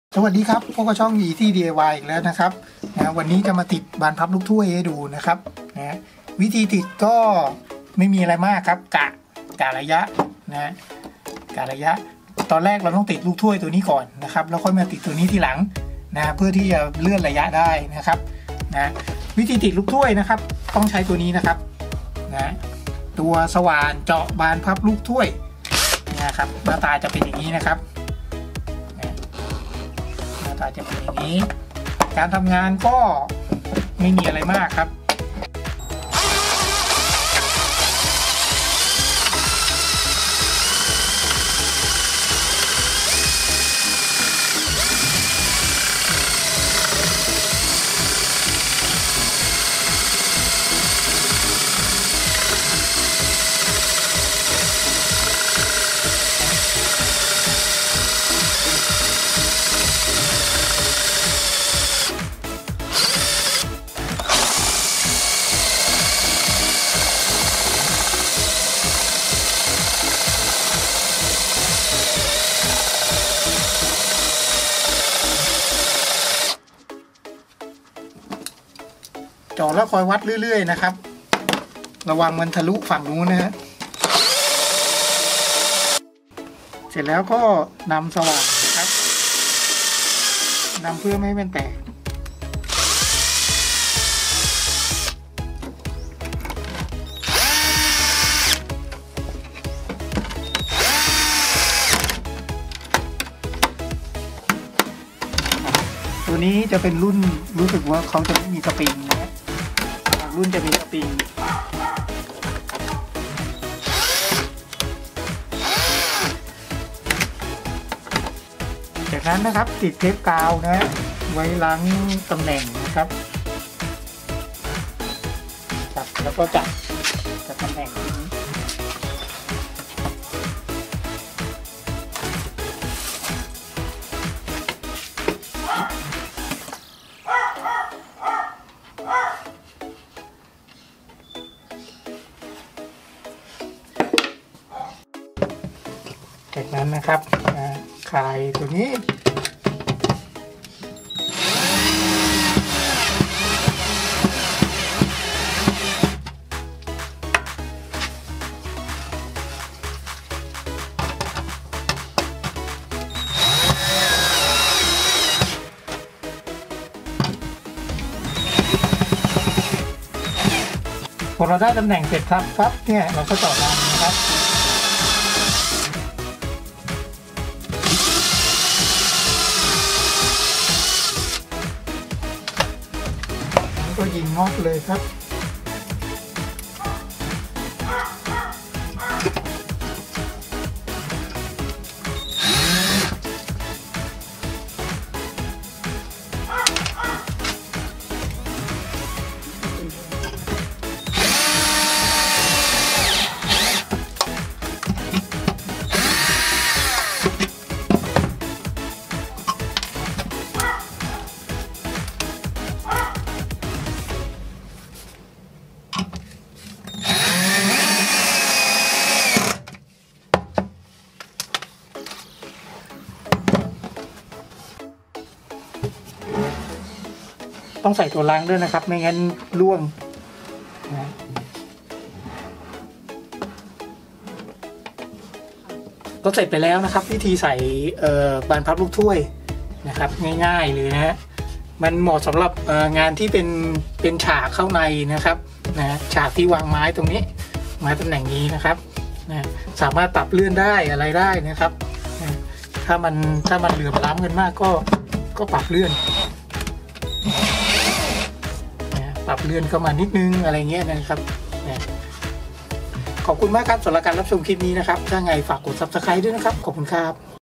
งานไม้ งานช่าง diy!!!แชร์เทคนิคการติดบานพับลูกถ้วย (สำหรับมือใหม่ )
สวัสดีครับวันนี้เอาเทคนิคการติดบานพับลูกถ้วย (สำหรับมือใหม่ ) พร้อมกับการเลือกอุปกรณ์ในการใช้ติดตั้งบานพับลูกถ้วย อย่างละเอียดเหมาะสำหรับคนชอบทำงานไม้เบื้องต้น
บานพับลูกถ้วย บานพับถ้วย หรือบานพับสำหรับงานเฟอร์นิเจอร์ ในท้องตลาดจะแบ่งลักษณะการติดตั้งร่วมกับหน้าบานไว้ 3 รูปแบบคือ ริมทับขอบ, กลางขอบ และในขอบ ซึ่งขนาดของถ้วยบานพับจะมีขนาดเส้นผ่านศูนย์กลางที่แตกต่างกัน คือ 40มม. 35มม. และ 26มม